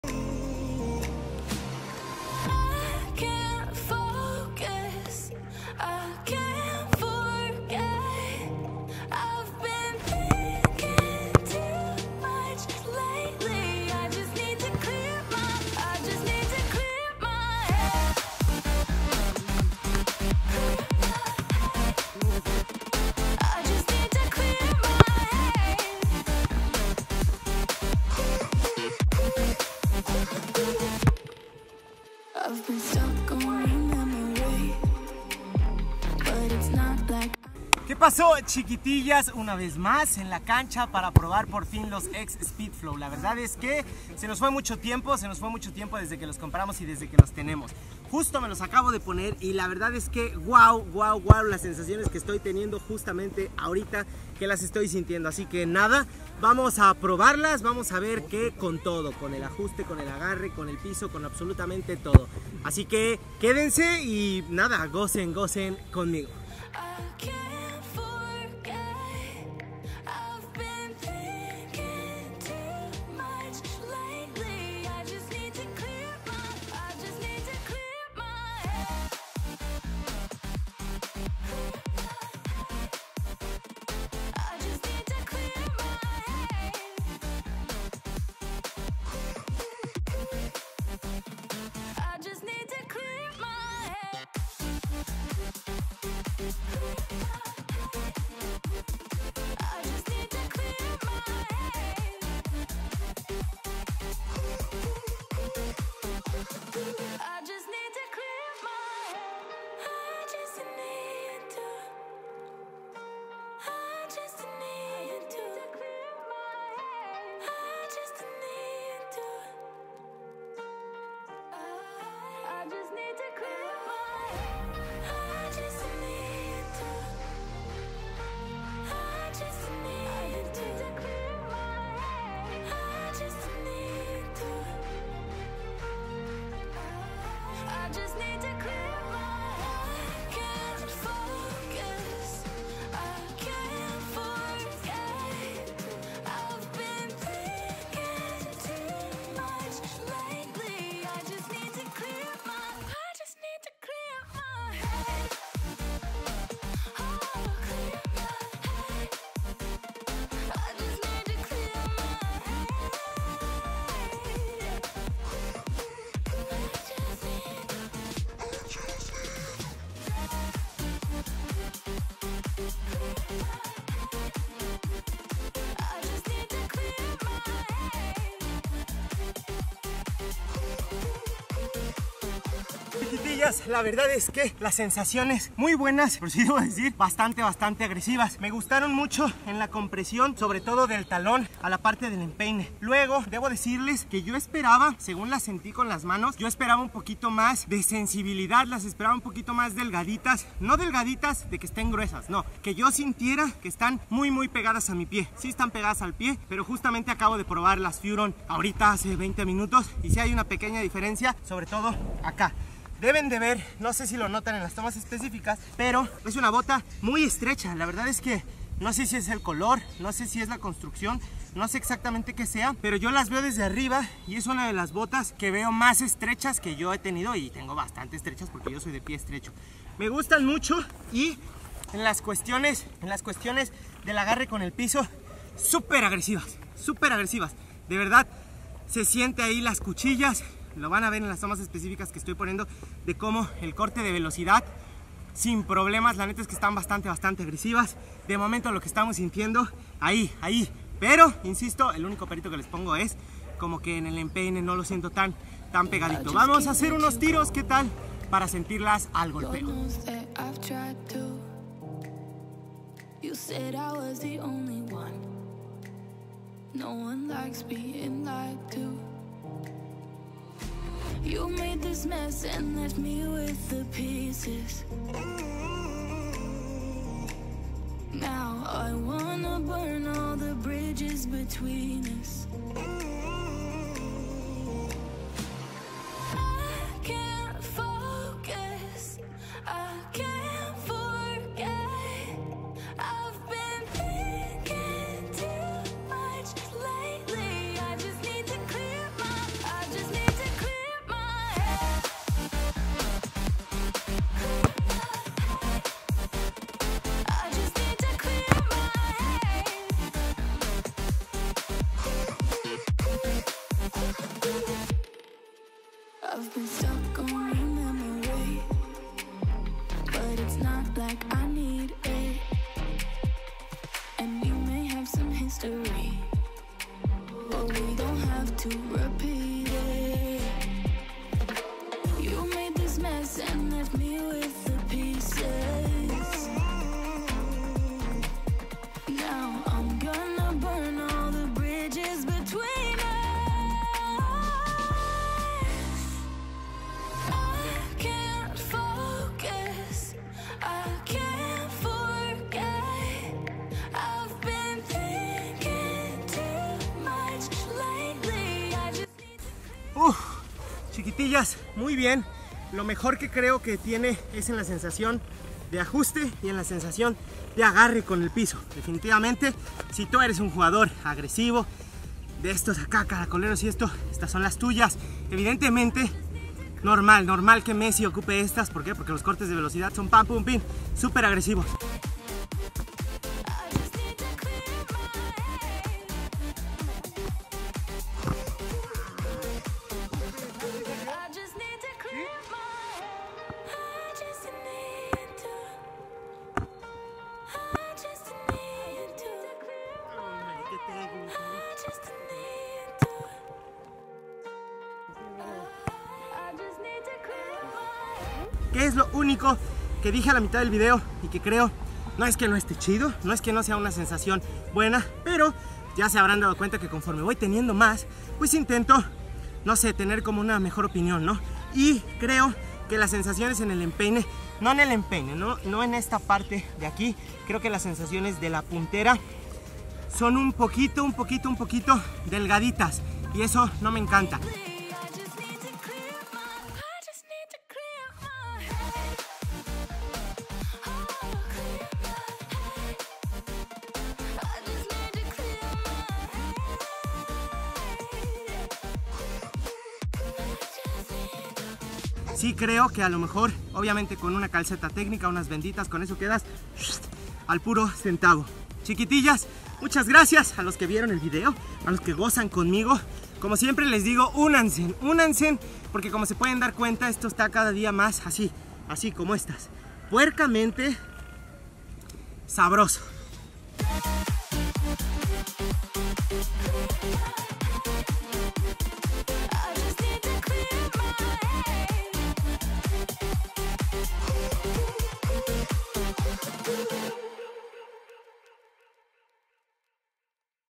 Las Speedflow de Messi en la cancha! ¿Qué tan buenas son? | Speedflow .1 en el campo
Chiquillas, pasó algo de tiempo (la verdad es que no fue planeado, solo sucedió y ni cuenta nos dimos) para que pudiéramos estar de nuevo en la cancha y estuviéramos probando otros tachos para revisar qué tal jalan.

Vamos a comenzar el año con los tacos de Lionel Messi 🤩 las mismas con las que ganó el último balón de oro.

Me encanta cómo se ven y desde que me las puse me gustaron mucho, hasta editando el video sabía que se iban a ver BIEN SABROSAS!

Pero, y en la cancha apá?
Échale ojito y cuéntame cómo las ves tú 🤘🏼